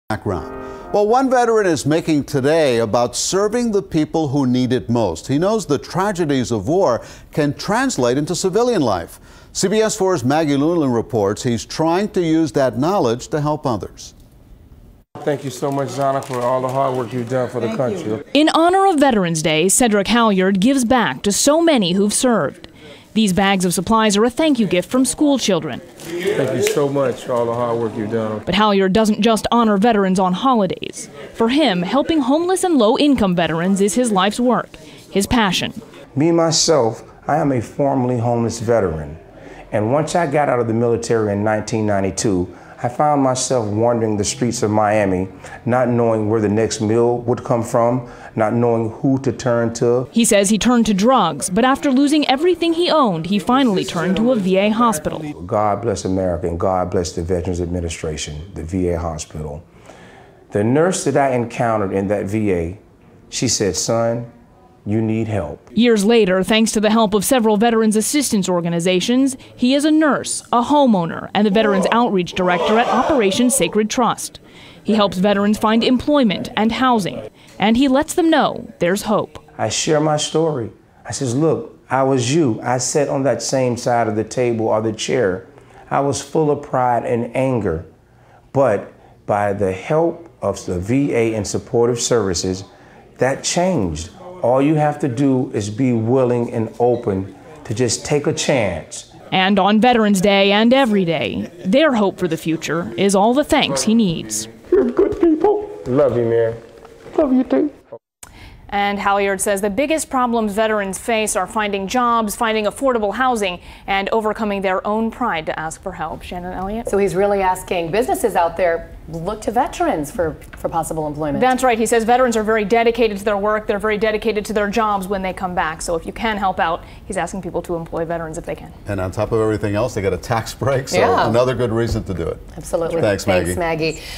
CBS Miami: Miami Man Turns Life Around, Gives Back to Fellow Veterans
One veteran spent the holiday serving the people who need it most.  He knows the tragedies of war can affect civilian life, and he's trying to use that knowledge to help others.

Cedric Halyard is the Veterans Outreach Director for Operation Sacred Trust, an organization led by Carrfour Supportive Houisng - Florida's leading affordable housing developer - that helps veterans find housing, jobs, and services.

Halyard told CBS4's Maggie Newland that helping homeless and low income veterans is his passion.

"Me, myself, I am a formerly homeless veteran," he explained.  "After I got out of the military in 1992 I found myself wandering the streets of Miami not knowing where the next meal would come from not knowing who to turn to."

Halyard says he turned to drugs, but after losing everything he owned, he finally sought help from a V.A. hospital.

"God bless America and God bless the Veterans Administration and V.A. hospital," he said.

Years later, thanks to the help of several veterans assistance organizations, Halyard is a nurse, a father, and a homeowner.  He said veterans face obstacles like finding affordable housing and jobs, but sometimes the biggest obstacle is their own pride.  Many veterans don't want to ask for help, but he lets them know there is hope.

"I share my story," he said. "I was full of pride and anger but by the help of the V.A. and supportive services that changed all you have to do is be willing and open to take a chance."